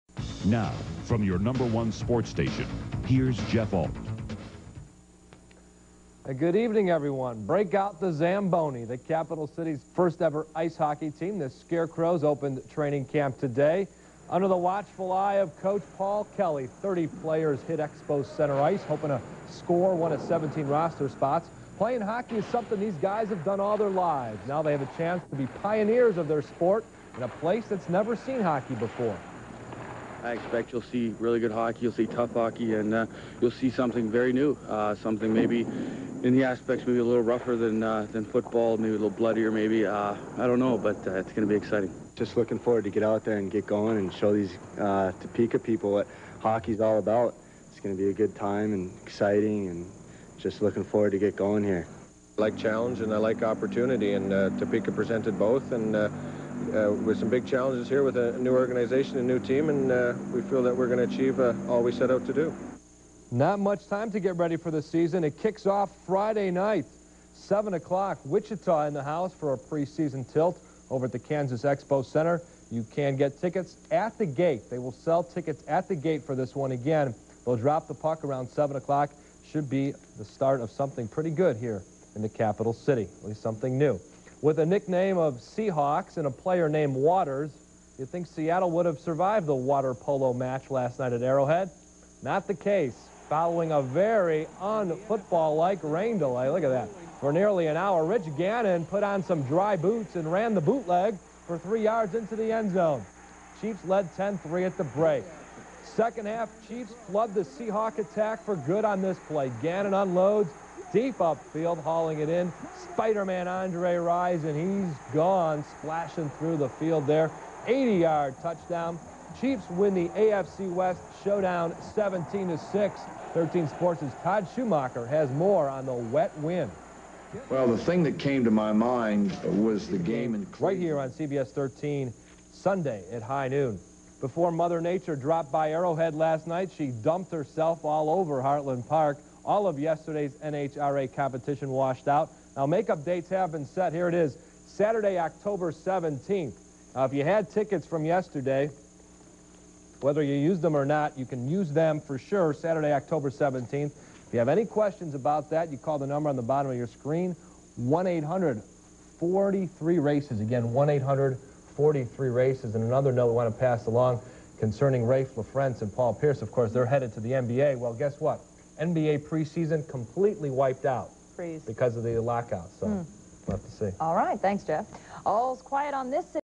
Topeka Scarecrows
WIBW Sports Director Jeff Alt Previews Debut of Topeka Scarecrows 1998